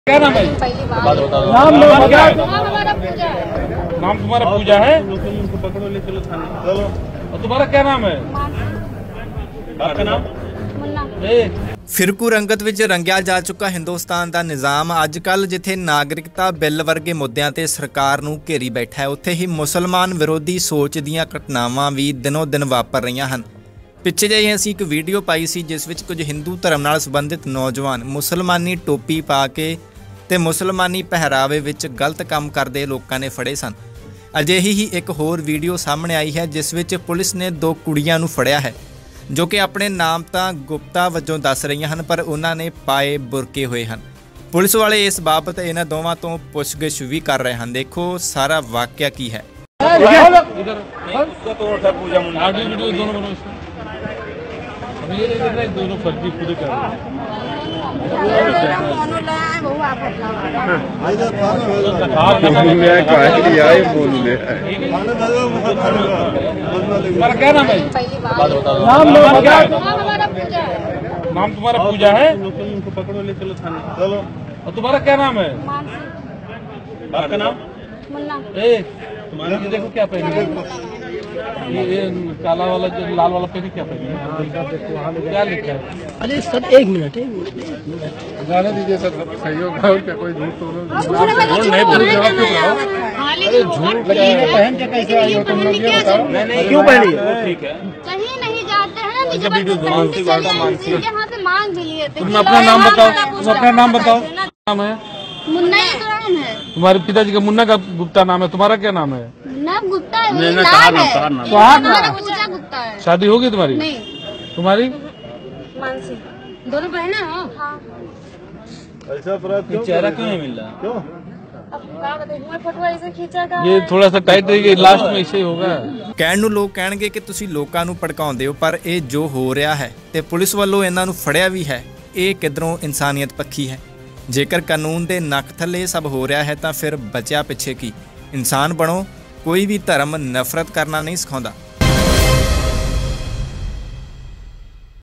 ਨਾਮ Pooja ਤੇ Mansi,ਪਾਏ ਹੋਏ ਬੁਰਕੇ | ਏਧਰ ਕੀ ਹੋਇਆ
ਫਿਰਕੂ ਰੰਗਤ ਵਿਚ ਰੰਗਿਆ ਜਾ ਚੁੱਕਾ ਹਿੰਦੁਸਤਾਨ ਦਾ ਨਿਜ਼ਾਮ ਅੱਜਕਲ ਜਿਥੇ ਨਿਗਰਿਕਤਾ ਬਿੱਲ ਵਰਗੇ ਮੁੱਦਿਆਂ ਤੇ ਸਰਕਾਰ ਨੂੰ ਘੇਰੀ ਬੈਠਾ ਹੈ ਓਥੇ ਹੀ ਮੁਸਲਮਾਨ ਵਿਰੋਧੀ ਸੋਚ ਦੀਆਂ ਘਟਨਾਵਾਂ ਦਿਨੋ ਦਿਨ ਵਾਪਰ ਰਹੀਆਂ ਹਨ। ਪਿੱਛੇ ਜਿਹੇ ਅਸੀਂ ਇੱਕ ਵੀਡੀਓ ਪਾਈ ਸੀ ਜਿਸ ਵਿਚ ਕੁਝ ਹਿੰਦੂ ਧਰਮ ਨਾਲ ਸਬੰਧਿਤ ਨੌਜਵਾਨ ਮੁਸਲਮਾਨੀ ਟੋਪੀ ਪਾ ਕੇ ਤੇ ਮੁਸਲਮਾਨੀ ਪਹਿਰਾਵੇ ਵਿਚ ਗਲਤ ਕੰਮ ਕਰ ਰਹੇ ਸਨ ਜਿਨ੍ਹਾਂ ਨੂੰ ਕੁਝ ਲੋਕਾਂ ਨੇ ਫੜਿਆ। ਅਜਿਹੀ ਹੀ ਇੱਕ ਹੋਰ ਵੀਡੀਓ ਸਾਹਮਣੇ ਆਈ ਹੈ ਜਿਸ ਵਿਚ ਪੁਲਿਸ ਨੇ 2 ਕੁੜੀਆਂ ਨੂੰ ਫੜਿਆ ਹੈ ਜੋ ਆਪਣੇ ਨਾਮ ਤਾਂ ਗੁਪਤਾ ਵਜੋਂ ਦੱਸ ਰਹੀਆਂ ਹਨ ਪਰ ਉਹਨਾਂ ਨੇ ਪਾਏ ਬੁਰਕੇ ਹੋਏ ਹਨ। ਪੁਲਿਸ ਵਾਲੇ ਇਸ ਬਾਬਤ ਦੋਵਾਂ ਤੋਂ ਪੁਛਗਿੱਛ ਕਰ ਰਹੇ ਹਨ। ਦੇਖੋ ਸਾਰਾ ਵਾਕਿਆ ਕੀ ਹੈ ?

ਕਹਿਣ ਨੂੰ ਲੋਕ ਕਹਿਣਗੇ ਕਿ ਤੁਸੀਂ ਲੋਕਾਂ ਨੂੰ ਭੜਕਾਉਂਦੇ ਹੋ ਪਰ ਇਹ ਜੋ ਹੋ ਰਿਹਾ ਹੈ ਤੇ ਪੁਲਿਸ ਵਲੋਂ ਇਹਨਾਂ ਨੂੰ ਫੜਿਆ ਵੀ ਹੈ,ਇਹ ਕਿਧਰੋਂ ਇਨਸਾਨੀਅਤ ਪੱਖੀ ਹੈ ? ਜੇਕਰ ਕਾਨੂੰਨ ਦੇ ਨੱਕ ਥੱਲੇ ਇਹ ਸਭ ਹੋ ਰਿਹਾ ਤਾਂ ਫਿਰ ਕੀ ਬਚਿਆ ਪਿੱਛੇ ? ਇਨਸਾਨ ਬਣੋ,ਕੋਈ ਵੀ ਧਰਮ ਨਫਰਤ ਕਰਨਾ ਨਹੀਂ ਸਿਖਾਉਂਦਾ।